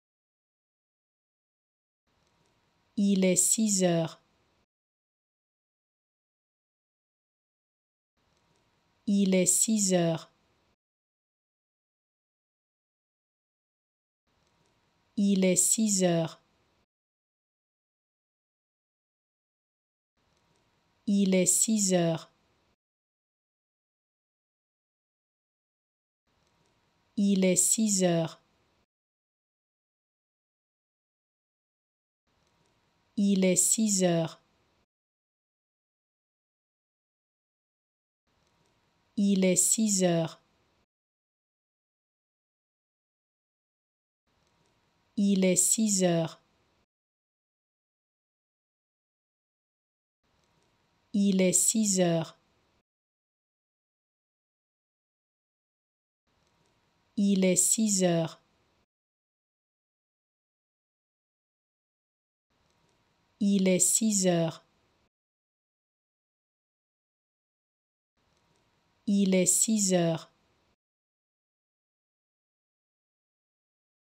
How to Pronounce ''Il est six heures'' (It's six o'clock) Correctly in French
Learn how to say and properly pronounce ''Il est six heures'' in French with this free pronunciation tutorial. Apprenez à prononcer des mots en français grâce à nos tutoriels.